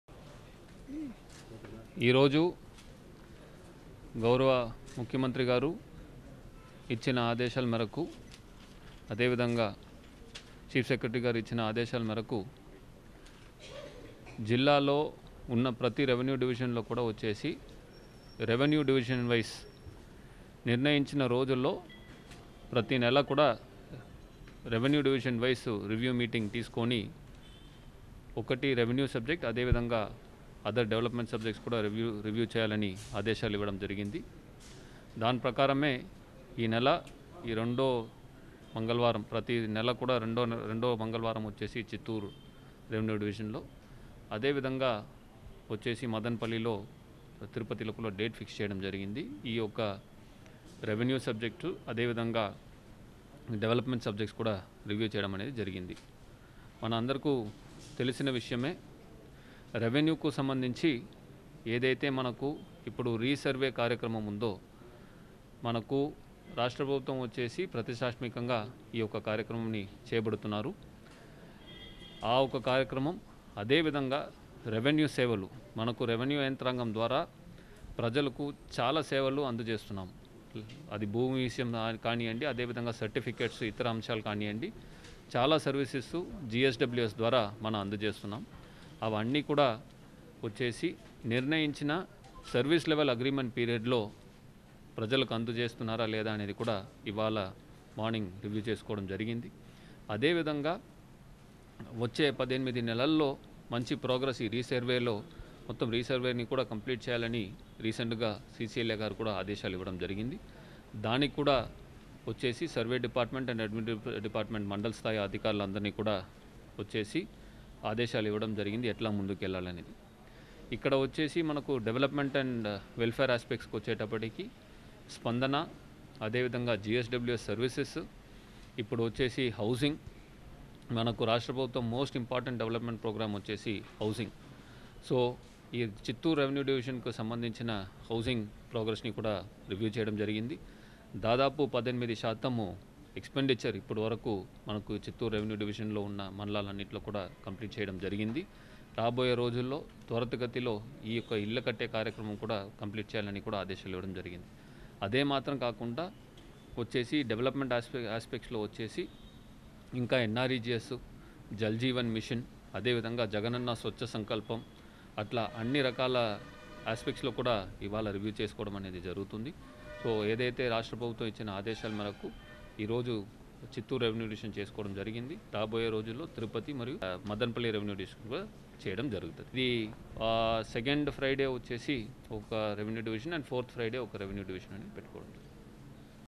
చిత్తూరురెవెన్యూడివిజన్ స్థాయి అధికారులతోసమీక్ష అనంతరంపత్రికా విలేఖరులతోమాట్లాడుతున్నజిల్లాకలెక్టర్
8.3.2022 చిత్తూరు జిల్లా పరిషత్ సమావేశ మందిరంలో  చిత్తూరు రెవెన్యూ డివిజన్ స్థాయి అధికారులతో సమీక్ష అనంతరం పత్రికా విలేఖరులతో మాట్లాడుతున్న జిల్లా కలెక్టర్ శ్రీ ఎం హరి నారాయణన్